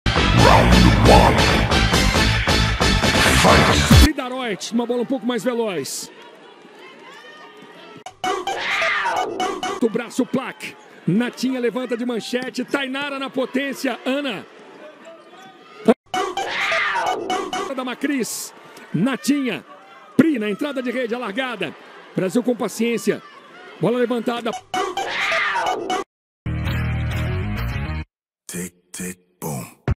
Quem é a Macris e quem é a Natinha? 💥💥
Macris faz altas defesas fazendo Natinha levantar de um lado pro outro nesse rally eletrizante com a Holanda - Mundial 2022